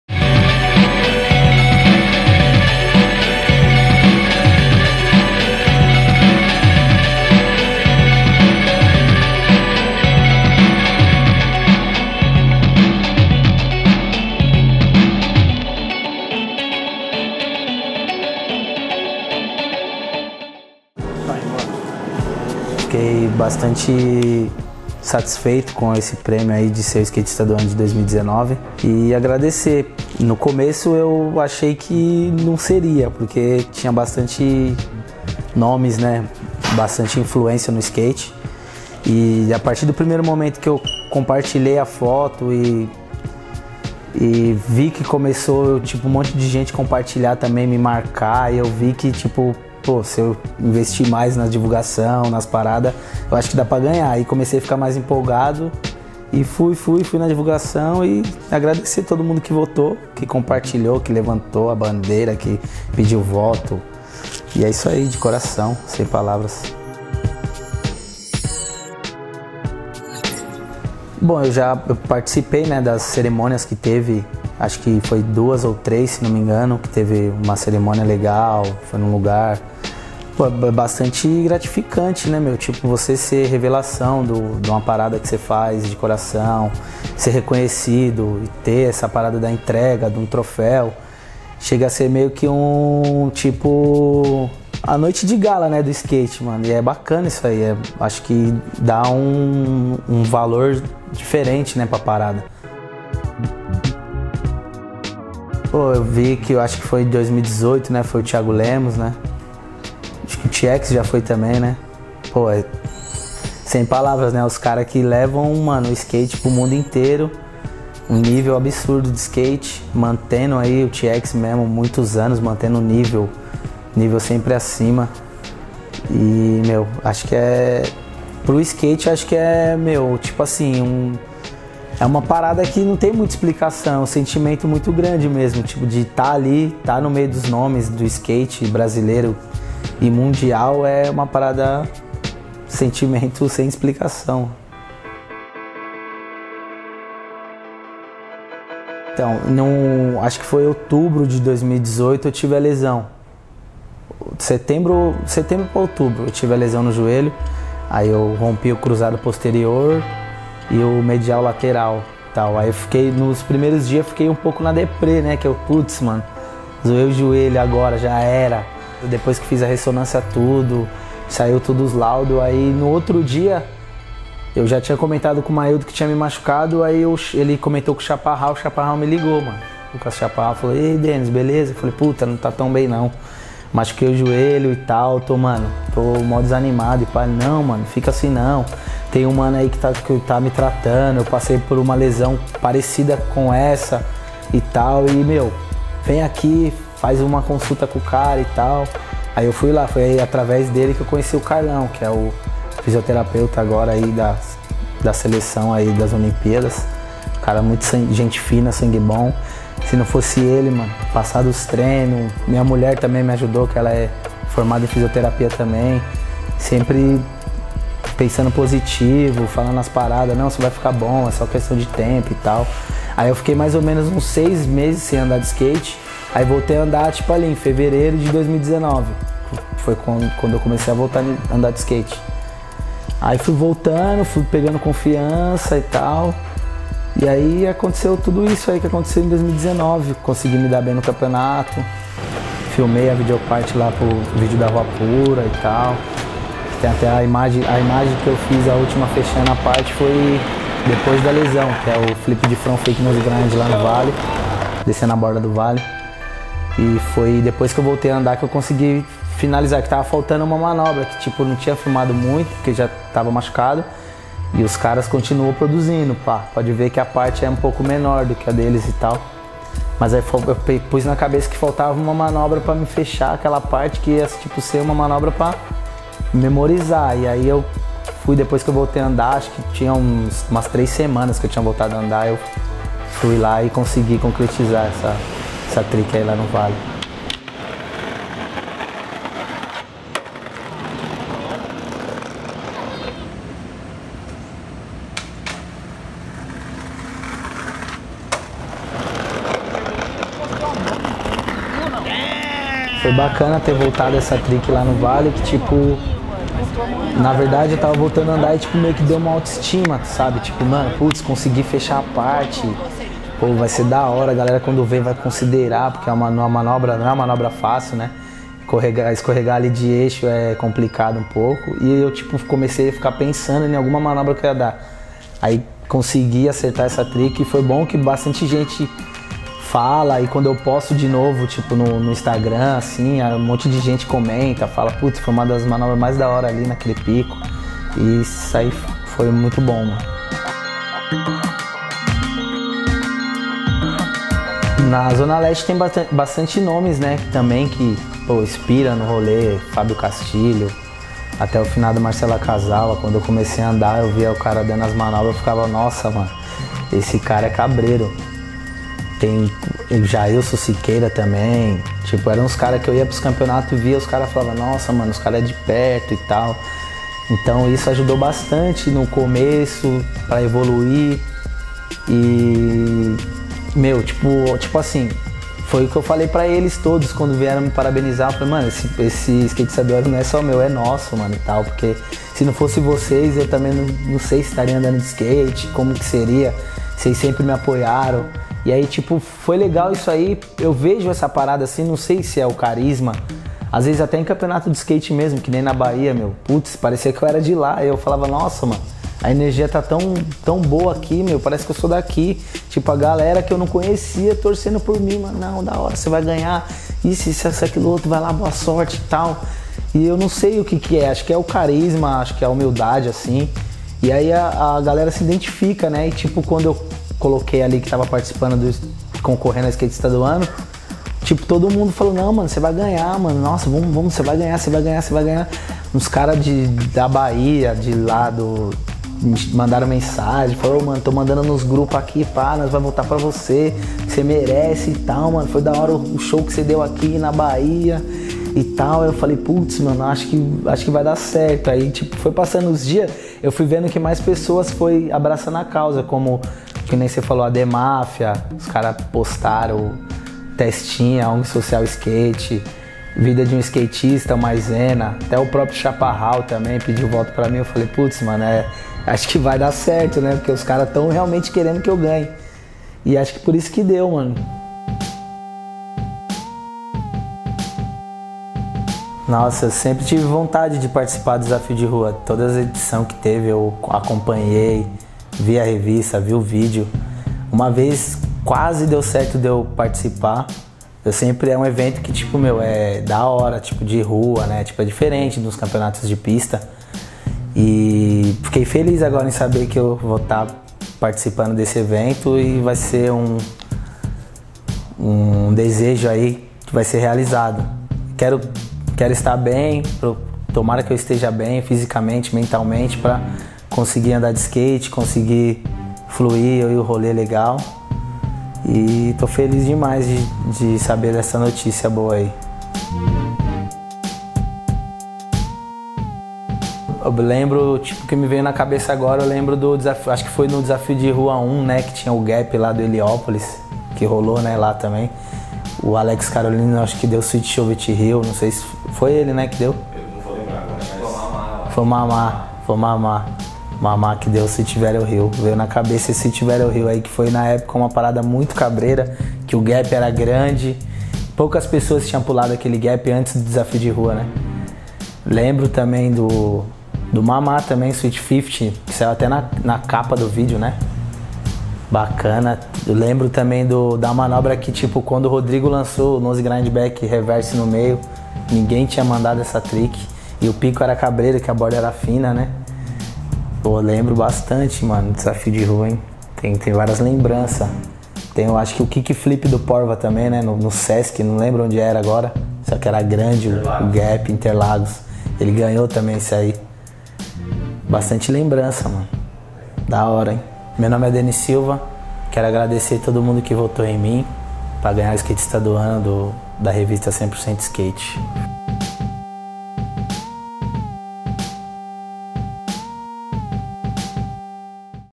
No AR #84: Denis Silva
Neste primeiro No Ar de 2020, o representante da Cidade Tiradentes Denis Silva fala sobre união, amigos, manobras, lesões, campeonatos e tudo que o levou a ser eleito o Skatista do Ano de 2019 no Troféu CemporcentoSKATE.

Denis também fala sobre o icônico Desafio de Rua, que a CemporcentoSKATE volta a realizar a partir deste ano, do qual ele é o primeiro skatista confirmado nas disputas.